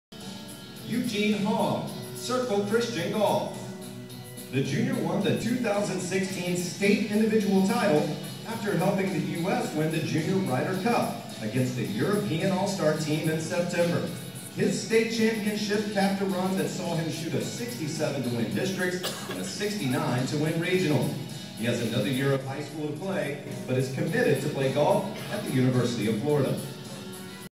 Eugene Hong 2017
Eugene Hong Boys Golfer of the Year, video from 2017 Orlando Sentinel Varsity Sports Annual Awards Show.